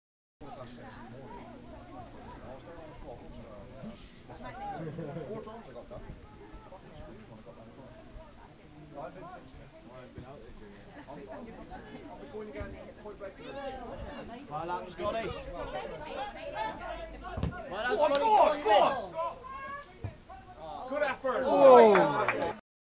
DUCHY Premier League game , St Stephen 1st Team 0 - 2 St Minver
St Stephen player Scott Hill goes close on 7 minutes.